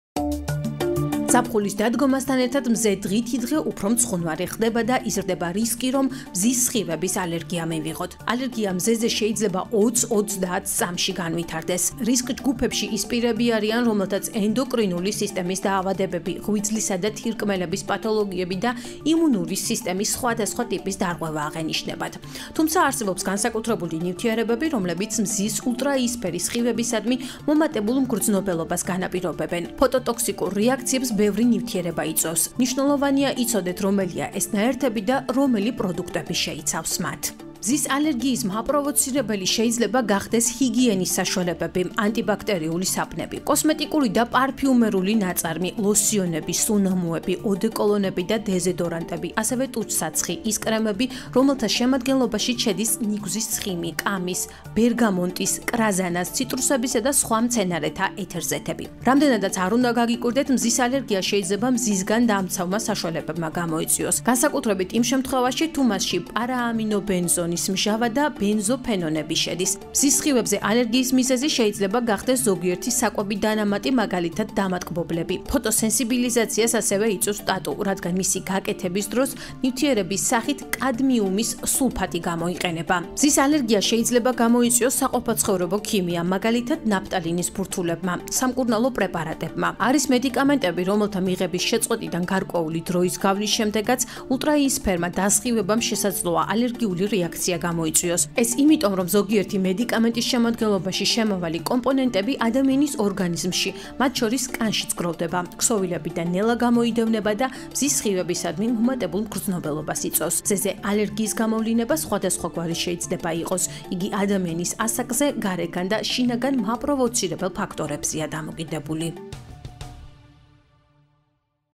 რა იწვევს მზის ალერგიას
მცხუნვარე მზეზე იზრდება რისკი რომ ალერგია მივიღოთ. არსებობს ისეთი ნივთიერებები, რომლებიც ამ ალერგიის პროვოცირებას უწყობენ ხელს, ამ ნივთიერებების შემცველ პროდუქტებს კი ჩვენ თითქმის ყოველდღიურად მოვიხმართ.